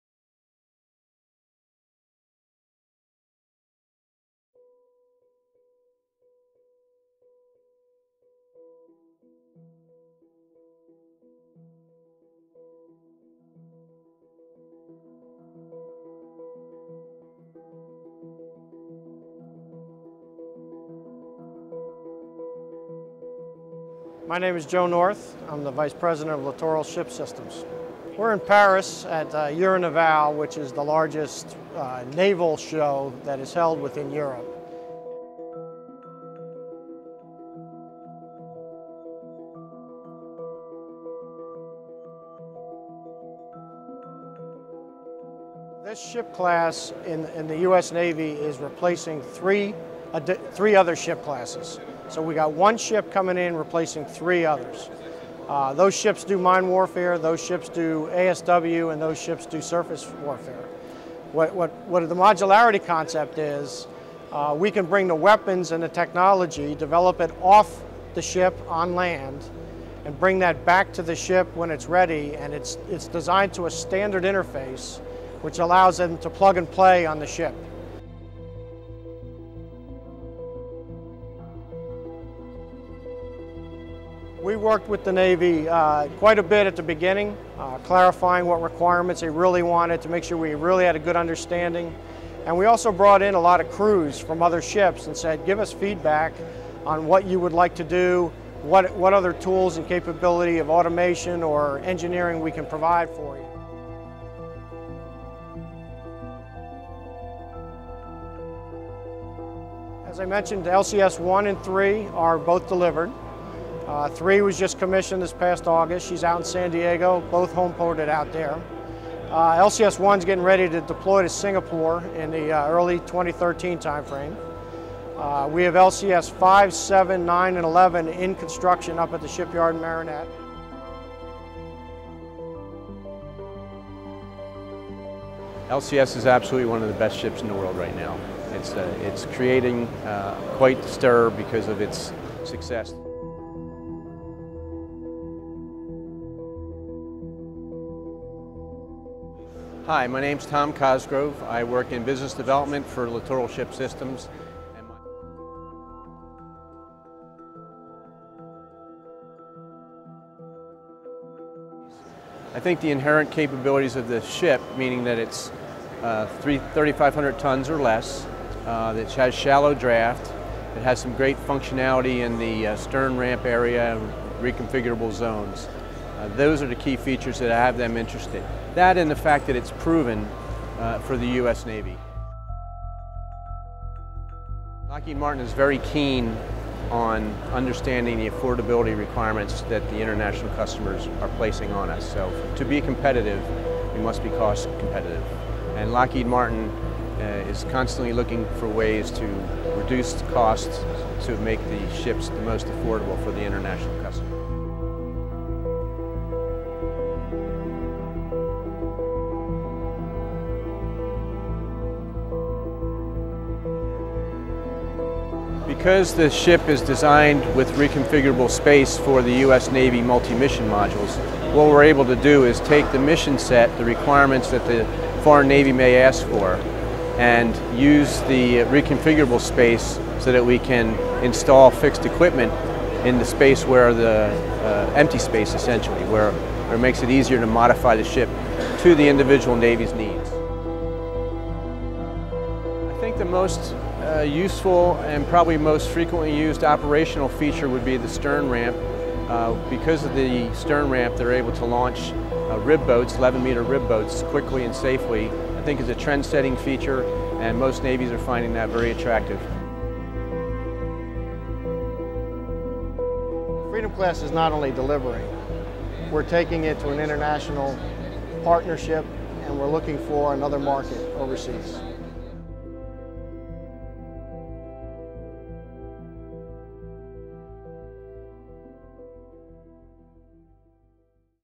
LCS at Euronaval 2012
At Euronaval 2012 in Paris, Lockheed Martin featured the Multi-Mission Combatant (MMC) based on its Littoral Combat Ship (LCS) design. Vice President of Littoral Ship Systems Joe North and MMC Business Development Director Tom Cosgrove discuss the progress of the LCS program and international opportunities.